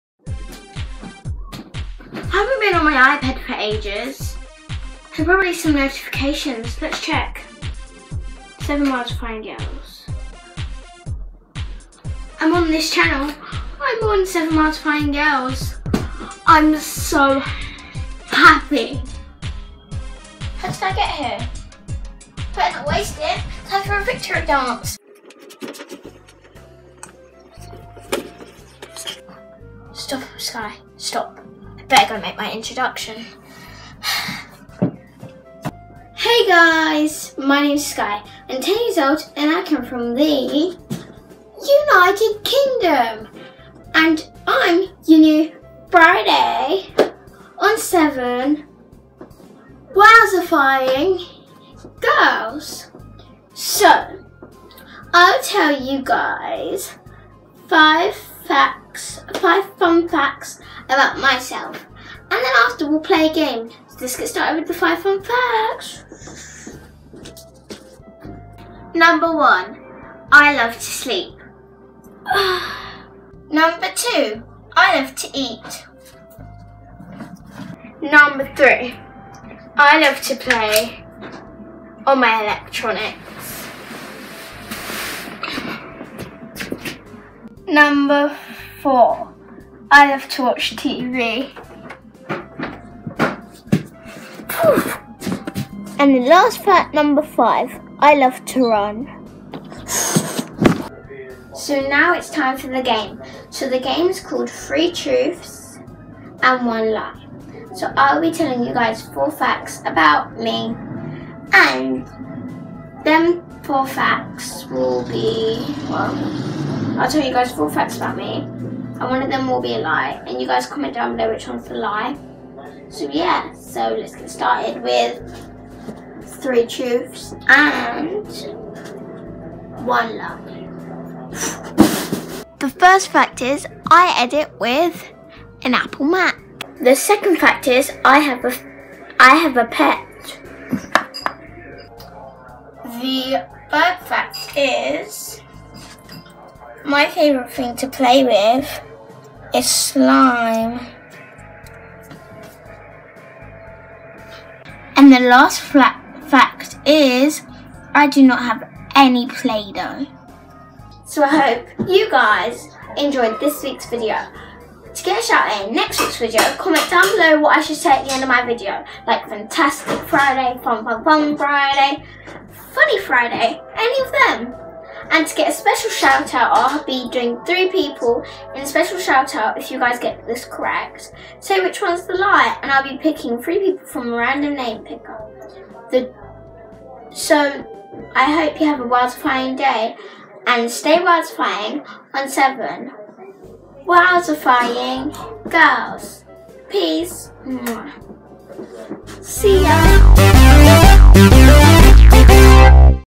Introducing your new Friday...
This Friday we are welcoming a new person into this wowzerfying family... SKYE!!